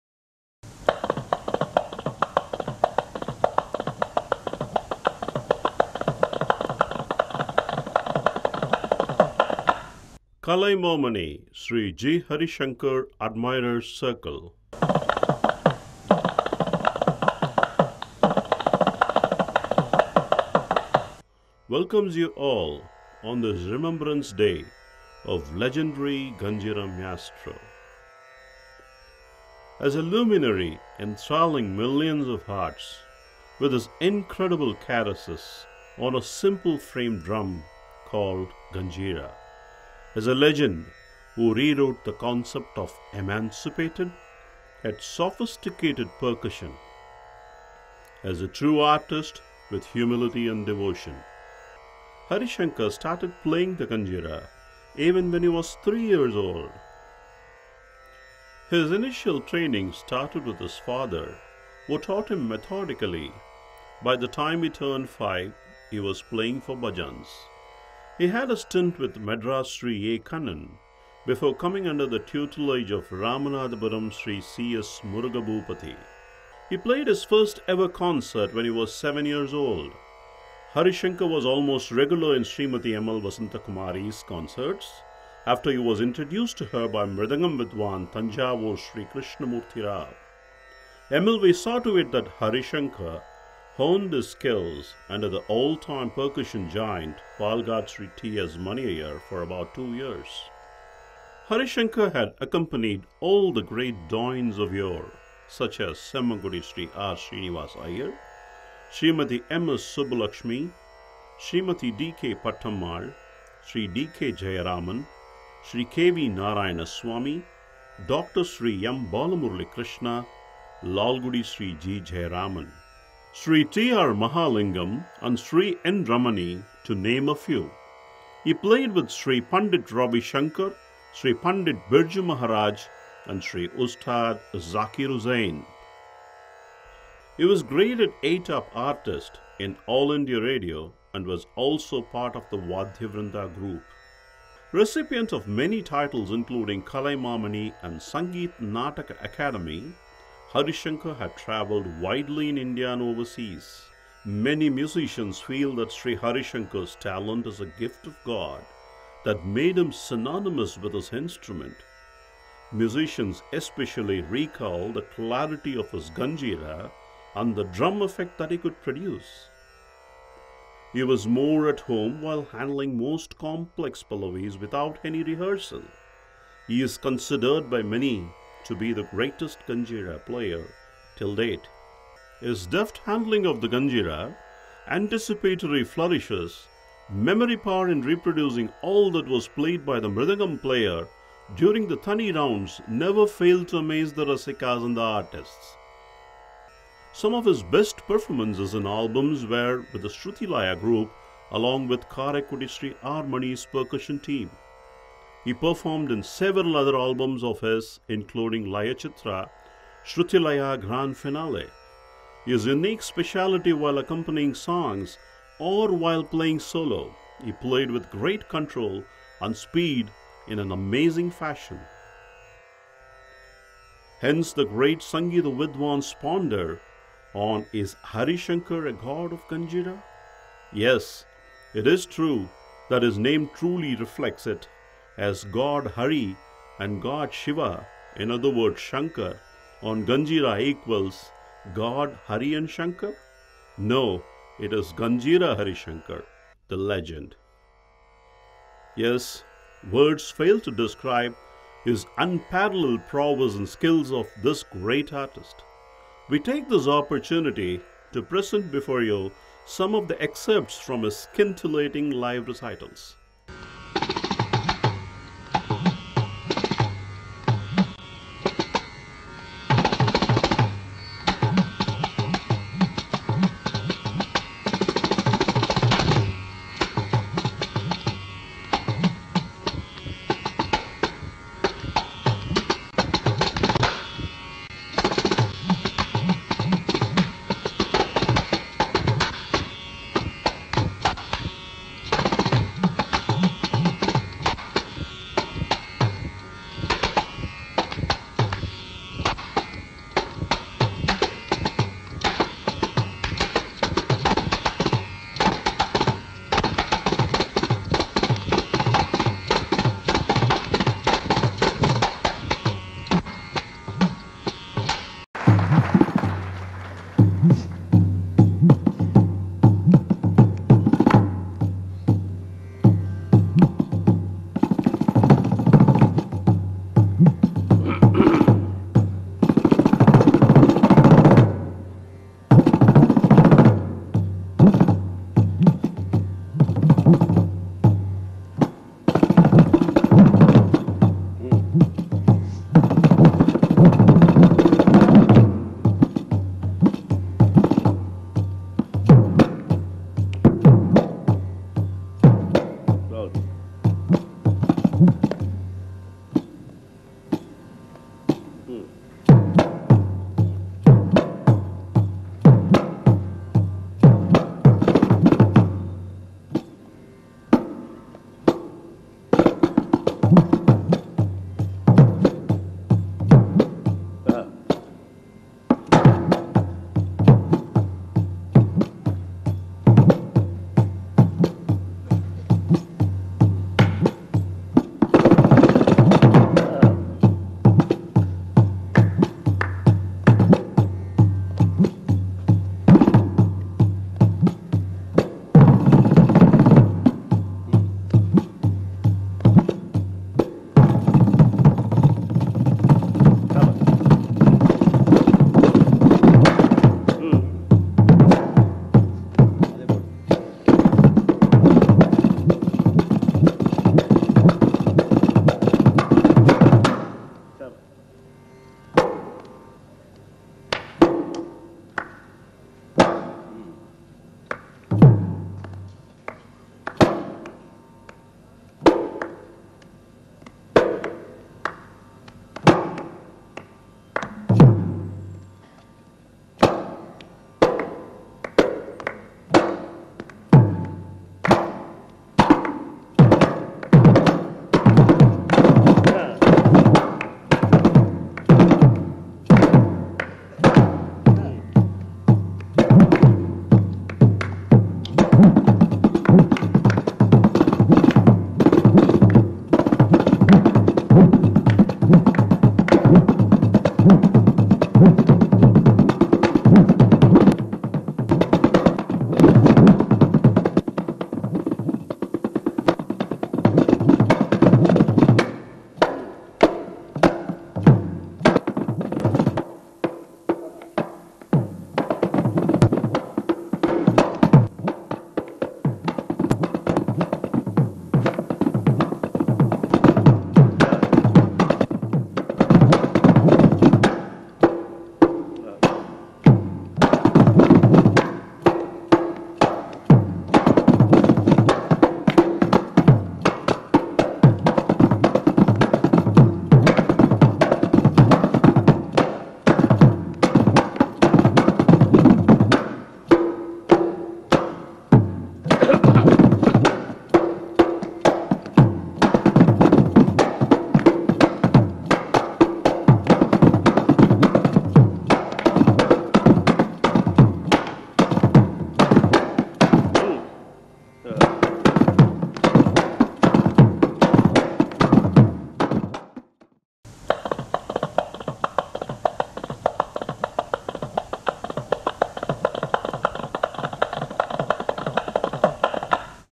Puvanur R V Iyer - Laya Vikadam Series -Episode - 124 - Documentary on Sri G.Harishankar
Documentary on Sri G.Harisankar (Kanjira Legend)  by Puvanur R Viswanathan